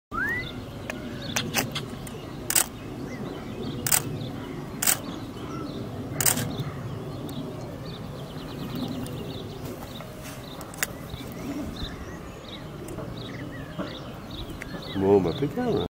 Bienvenue au petit lama !
Premiers pas du lama au Parc d'Isle